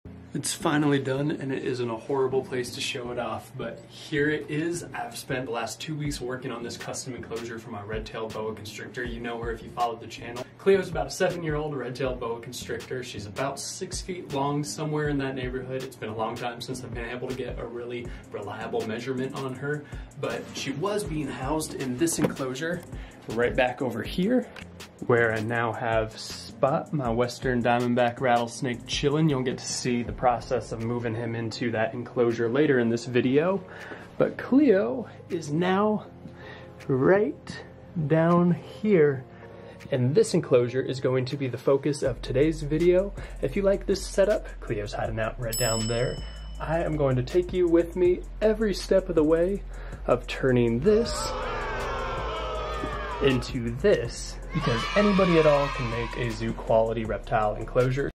Revealing My Boa’s New Stump! 🐍 DIY Reptile Habitat!
How to build a zoo quality reptile enclosure!

🦎🐍 Other Socials 🐊🐢

Herpin Hippie LLC.